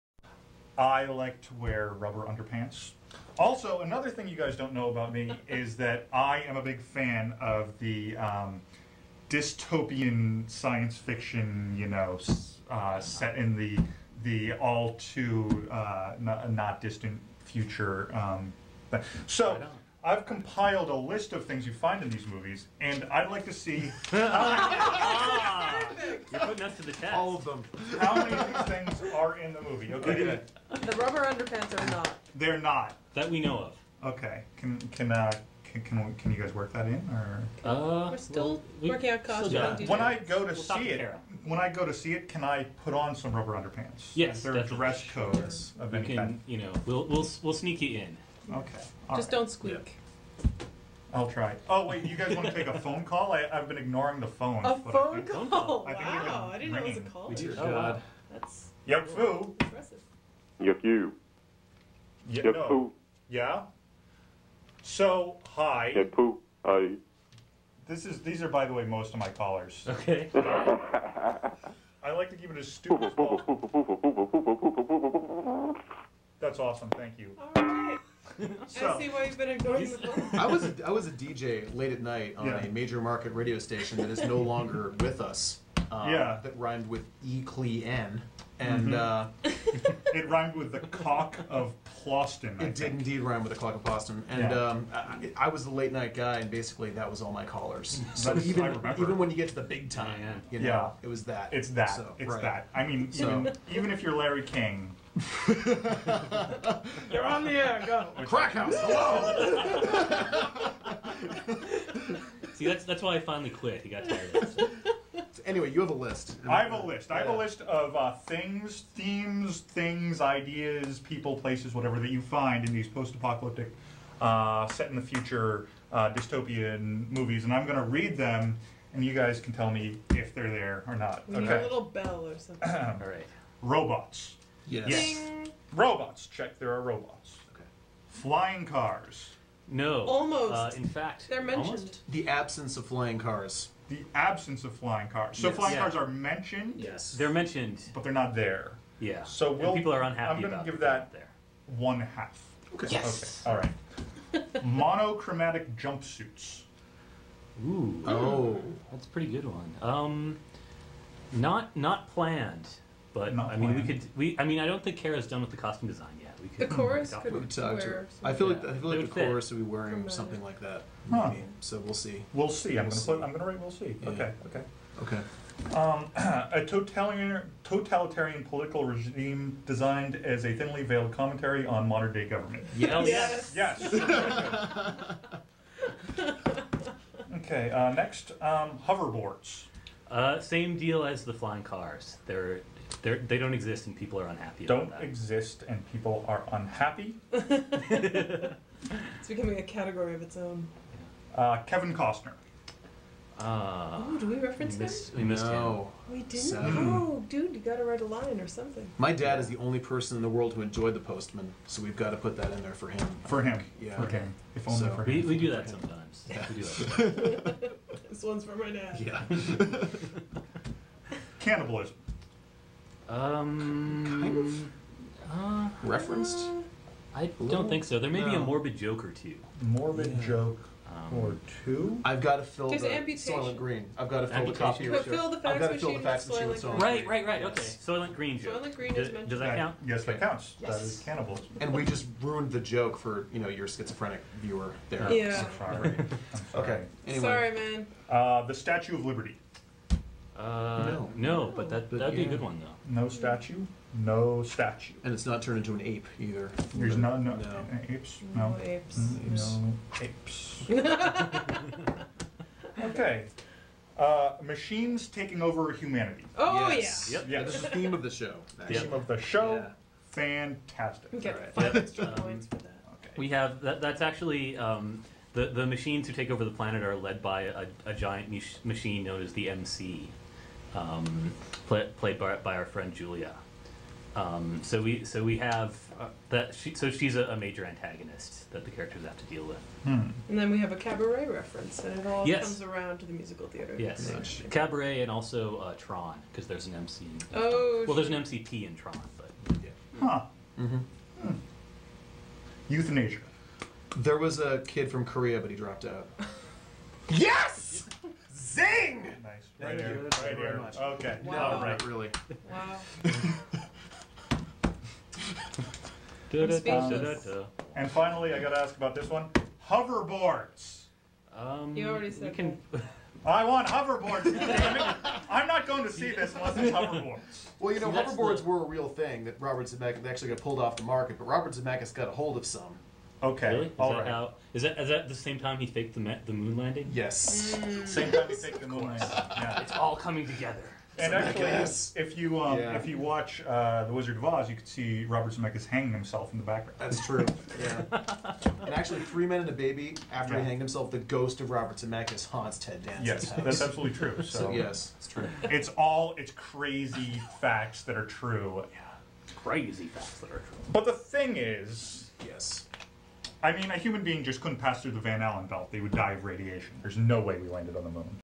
BeLive Interview: Monochromatic Jumpsuits
Will interviews Andy, Carl, and Kamela about "2010: Our Hideous Future: The Musical!"  Rubber-pantsed Will tests just how dystopian this futuristic-dystopian musical really is, and Andy reminisces about his days in radio.  The play runs August 20-28; tickets are available now